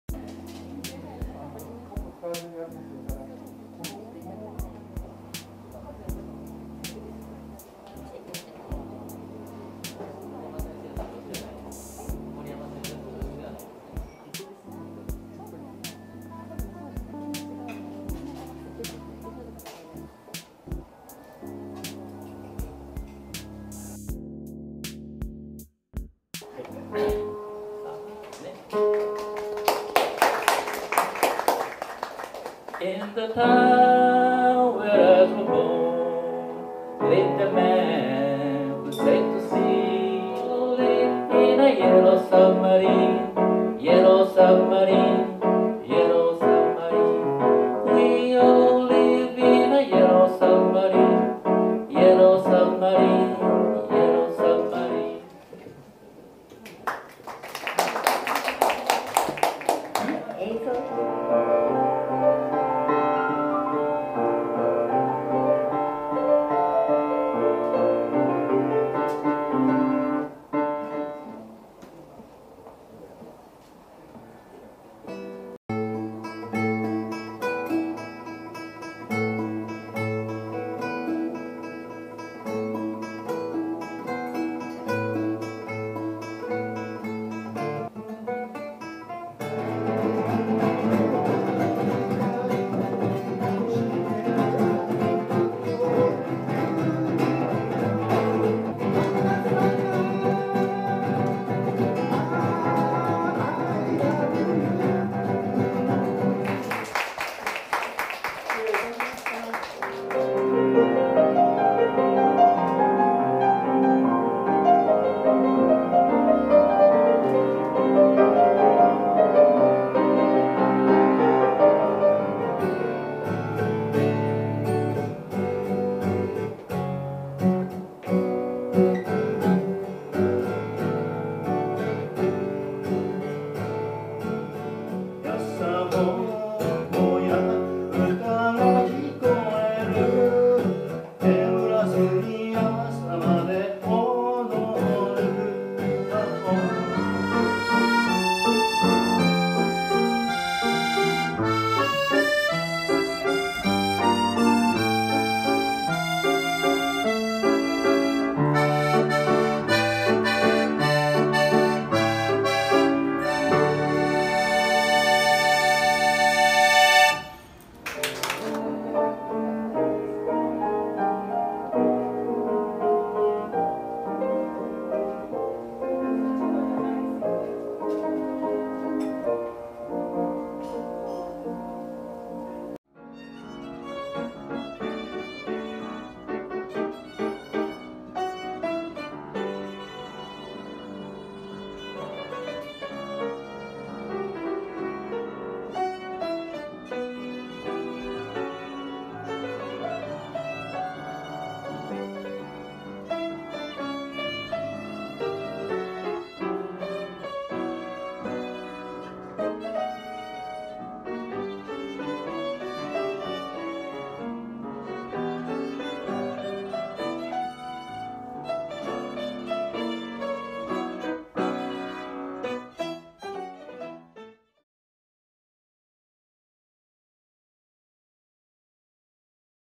大人のギター＆ピアノ『ミニ発表会』ダイジェスト版 atモンターニュ石原店
「大人、やば〜！」
「（大人の）皆さん、格好良かったです！」

と、子どもさんで唯一参加くださった
Sちゃんとご家族の方からご感想をいただきました！！
２年ぶりの昼間の大人の発表会です。

本当に音楽することが楽しい！という皆さんが
少人数ではあるものの集まっての発表会。

ギターやピアノソロだけでなく、
歌あり、ヴァイオリンやビオラあり、アコーディオンあり！
バラエティに富んでいて濃厚な時間を過ごせました。

ご参加いただきました皆様、ありがとうございました。
音楽へのピュアな思いが伝わる会となったと思います。

◎ 講師
ギター　泉乃正和
ピアノ　森山（泉乃）雪子

◎ピアノ & 作編曲（Piano Com.& Arr.）
森山雪子（Yukiko Moriyama）　＊ピアノの先生もしています♪

◎癒やしの音色『森といずみ』
音楽制作・生演奏・音楽教室（Healing music production)